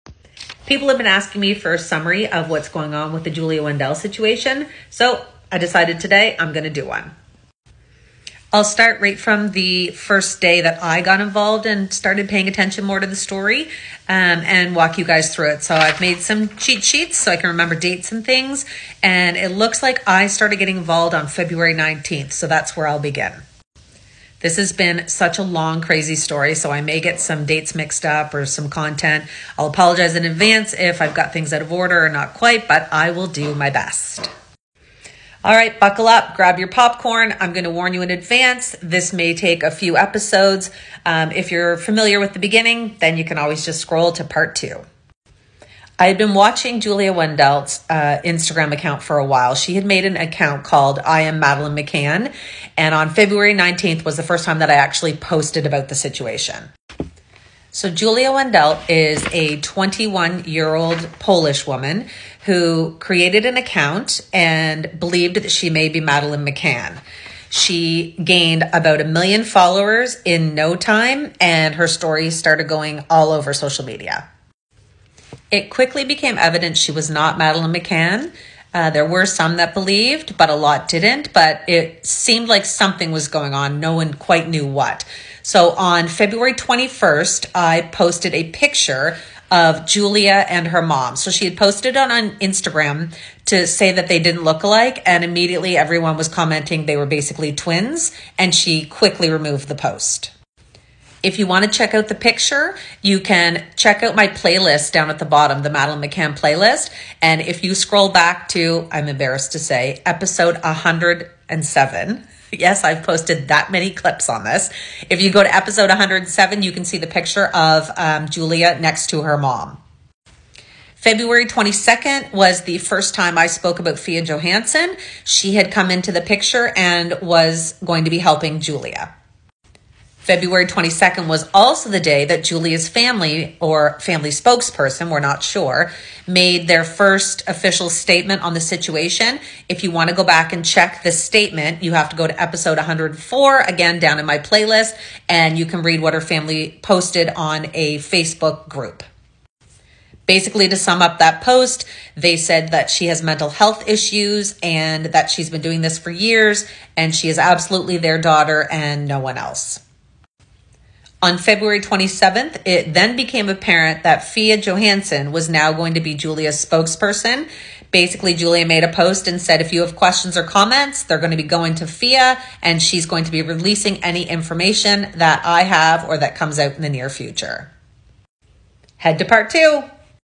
Part 1 - The story of Julia Wendell broken down
Part 1 - I have put together a summary of Julia Wendell, and how we've gotten to what's happening today.